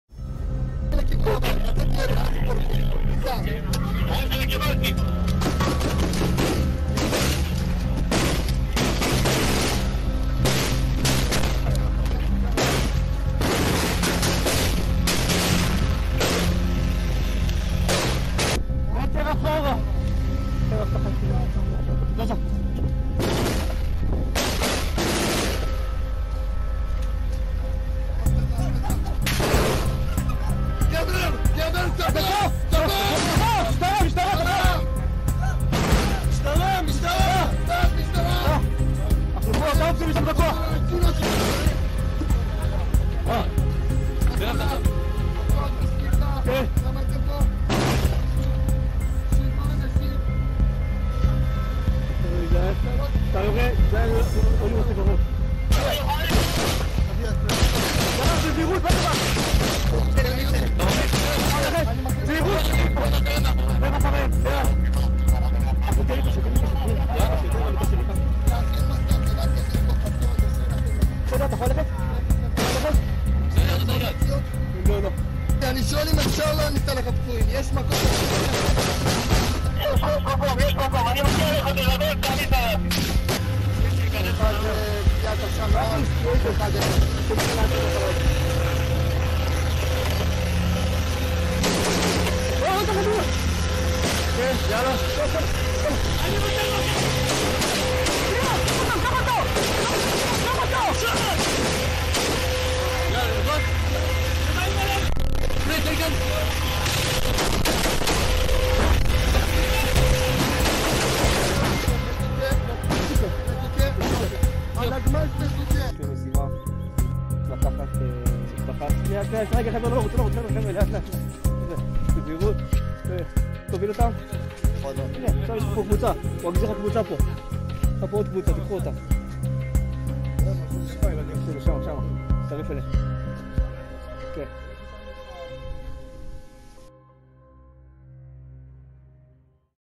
Melihat dari Dekat Kengerian Perang Israel dan Hamas | Liputan6
Liputan6 - Melihat dari Dekat Kengerian Perang Israel dan Hamas | Liputan6

Perang hebat masih berkecamuk antara militer Israel dan militan Hamas. Sebuah rekaman dirilis polisi Israel yang menunjukan suasana mencekam baku tembak antara pasukan Israel dan kelompok Hamas.